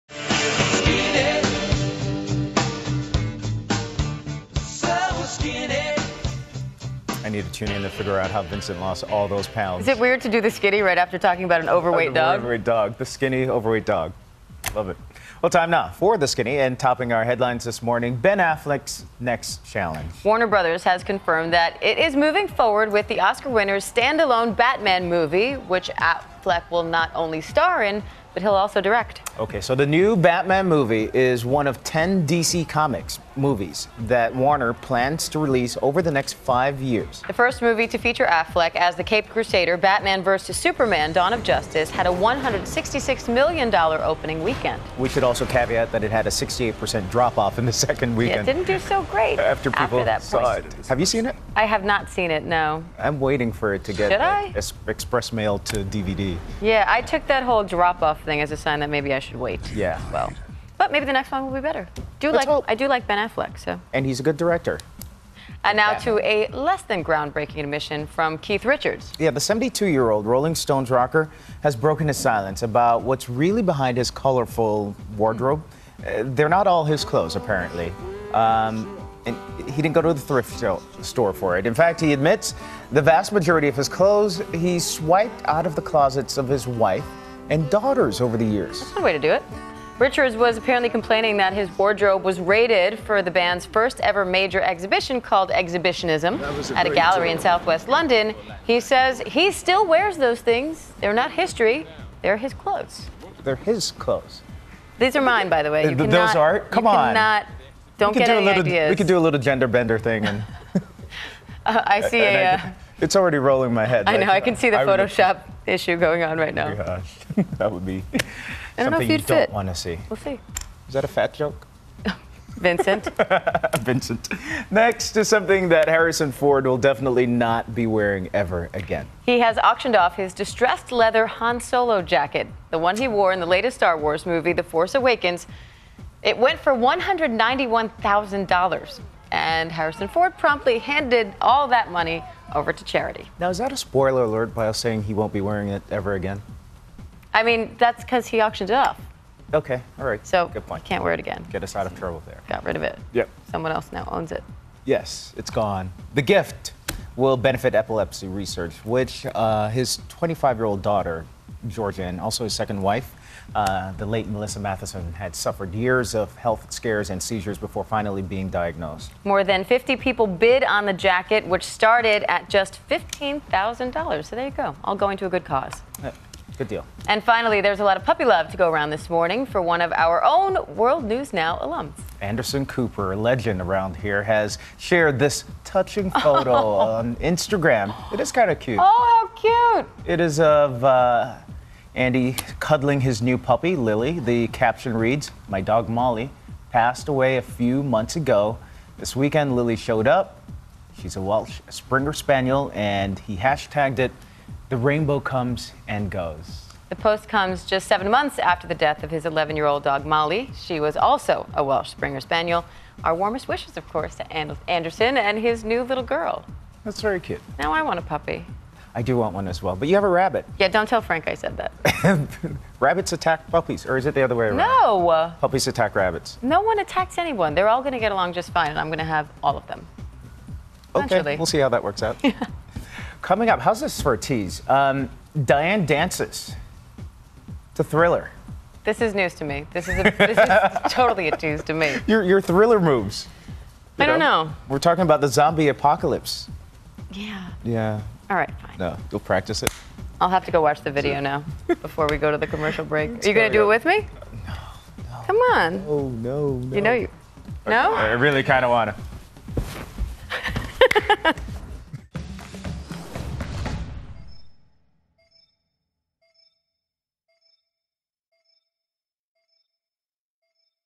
Ben Affleck Will Direct, Star In Next Batman Movie Warner Bros. Confirms
Ben Affleck, the Oscar award-winning actor, is set to the star and direct the next stand-alone Batman movie.